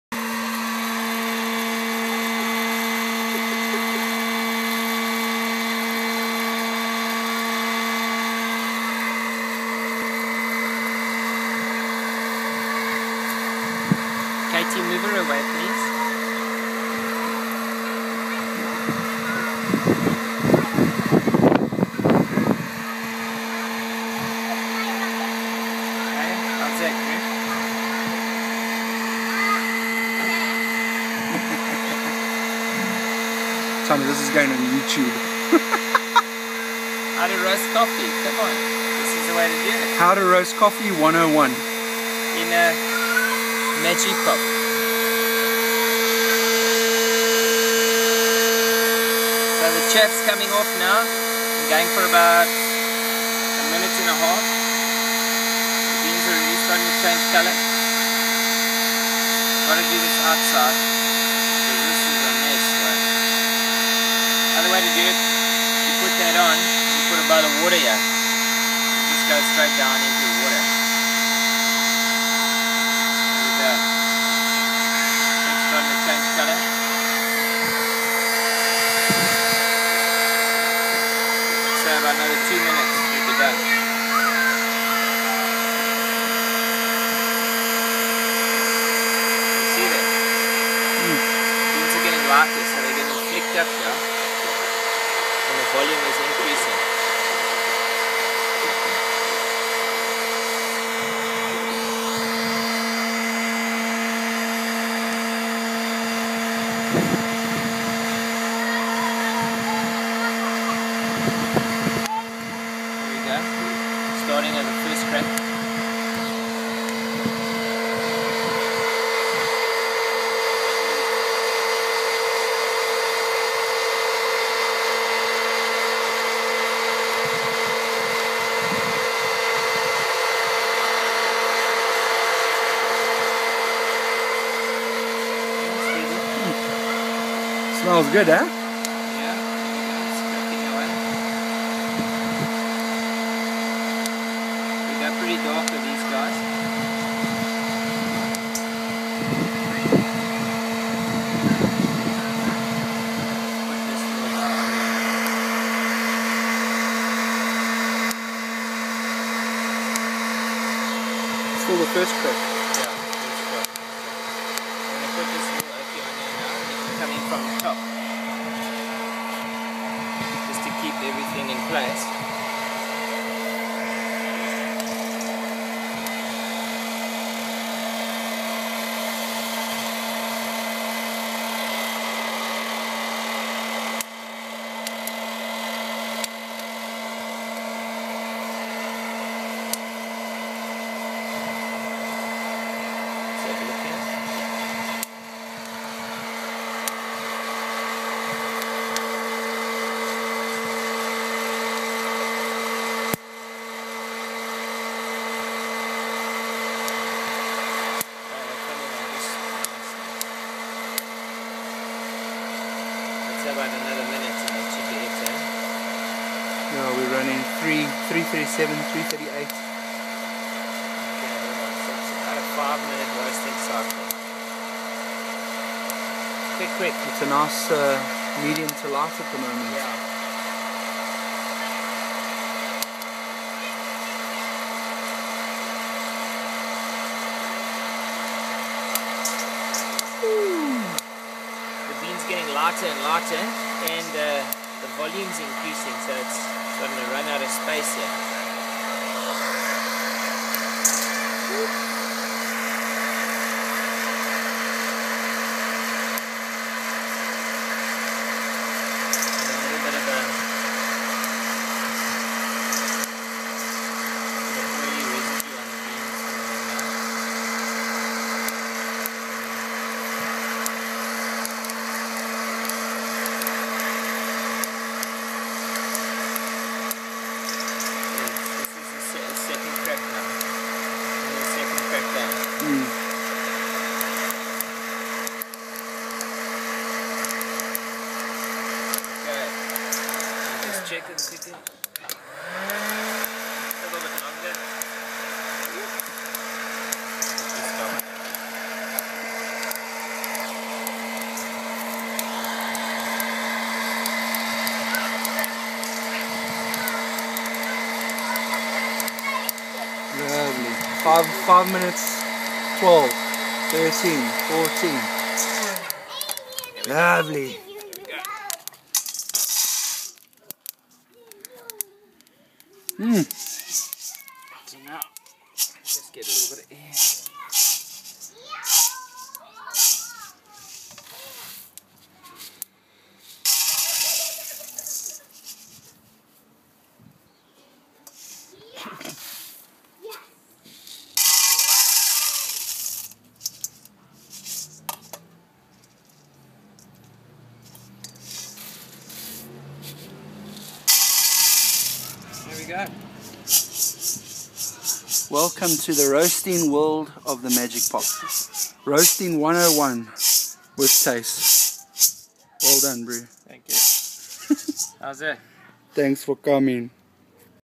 Coffee Roasting with Thys & Shaun the Coffee Guys.
Roasting coffee in a pop corn roaster. What an awesome cup of coffee this roast made...